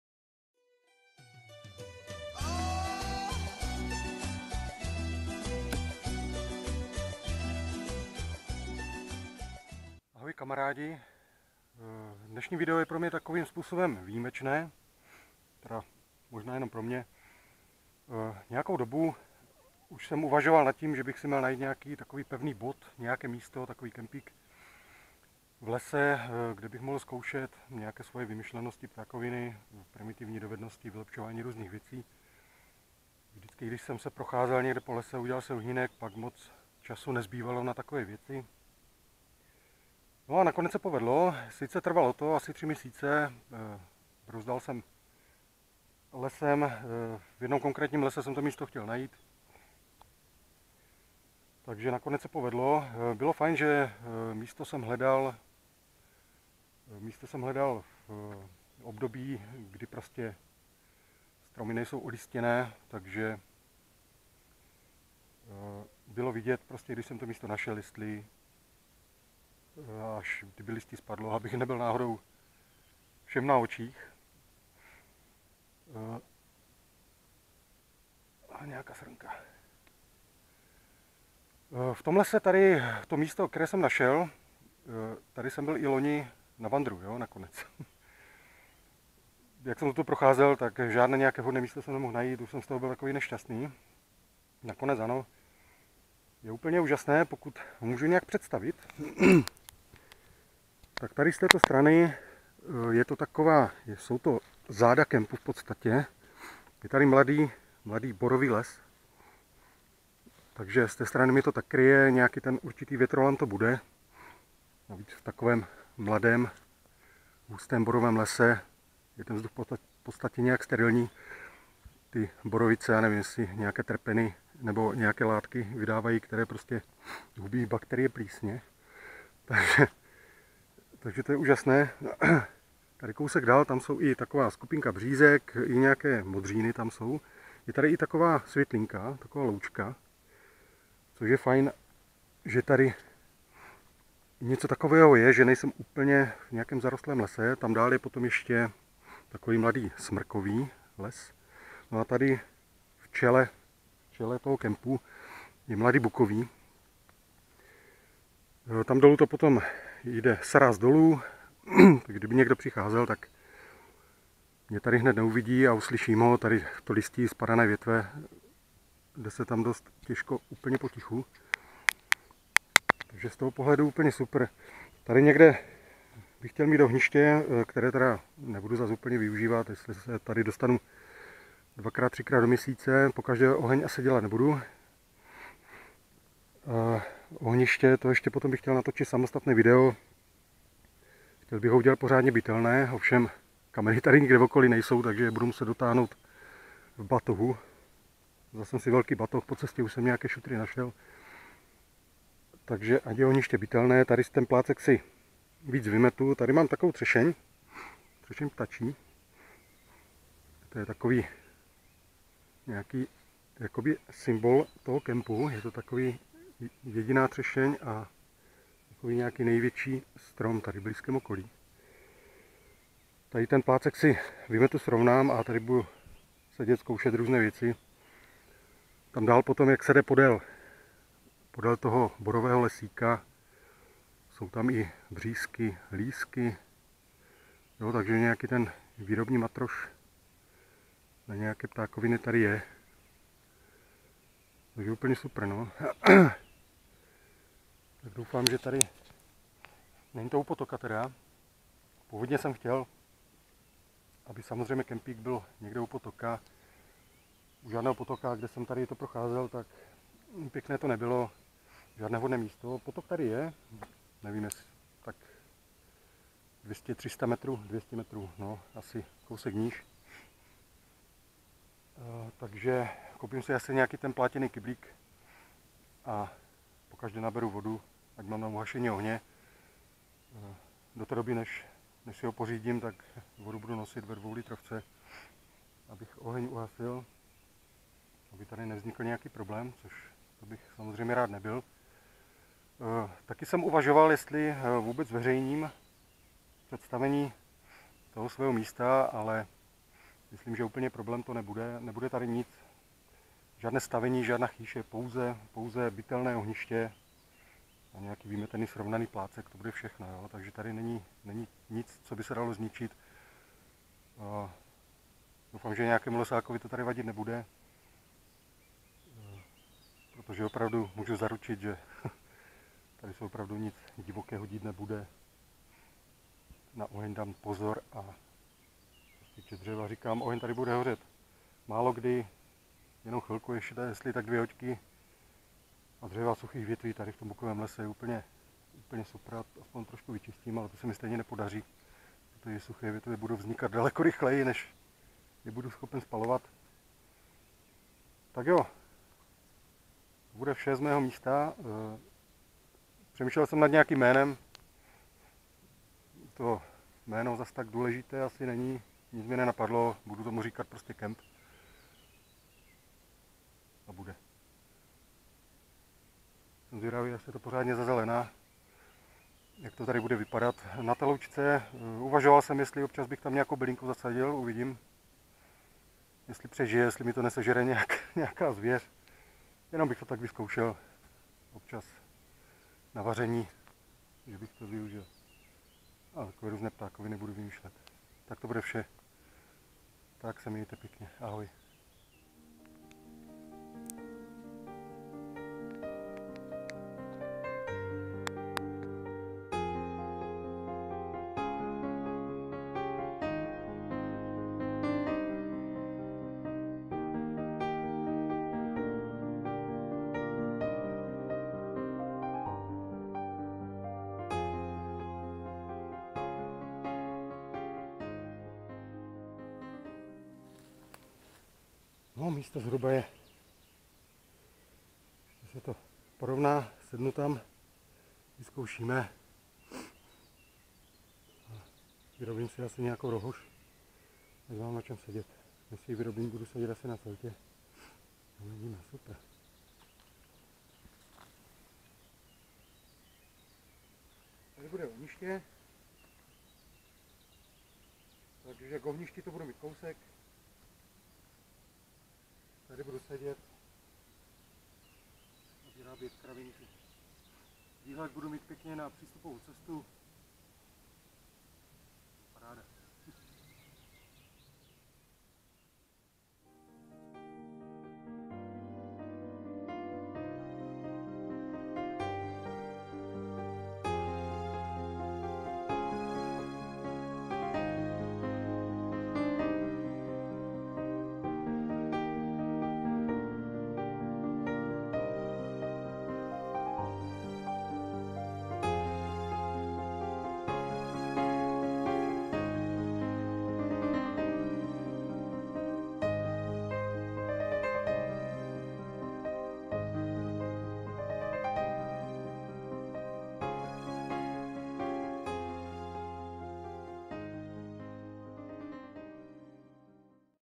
Představení Kempu - můj nový pevný bod uprostřed přírody :-).
Tak jsem si po třech měsících hledání našel své pevné místo uprostřed přírody, kde se budu vracet :-).